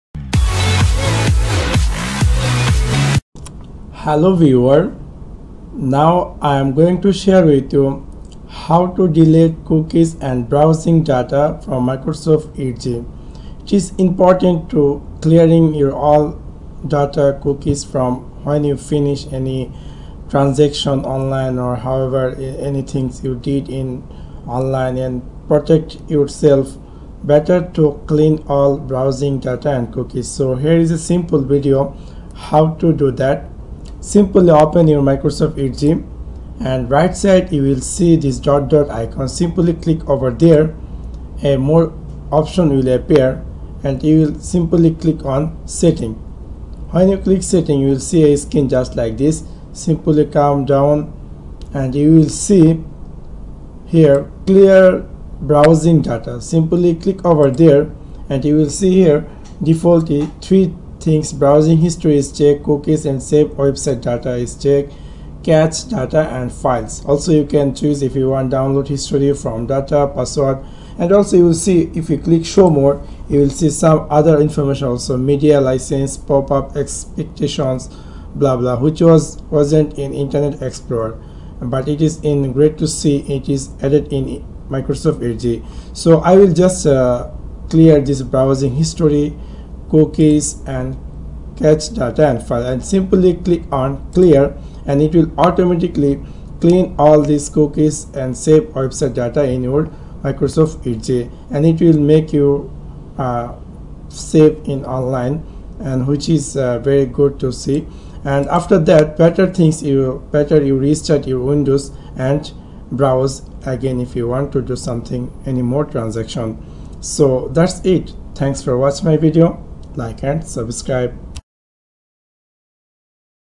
Windows 10 Tips And Tricks How To Clear Browsing Data Cookies After Banking Or Online Shopping
After banking or Shopping online here is how you can delete cookies and tracking data to have safe browsing